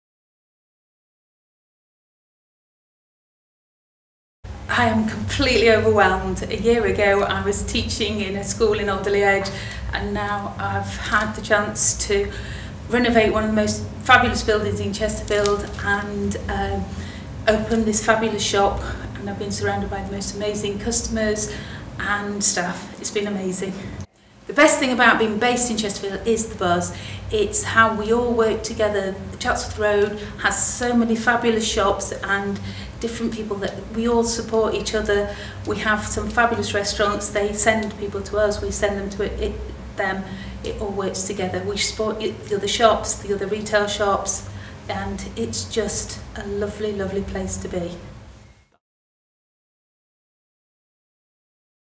Dotique winner of Best New Store at Chesterfield Retail Awards 2018
Dotique were the winners of the best new store at the Chesterfield Retail Awards 2018.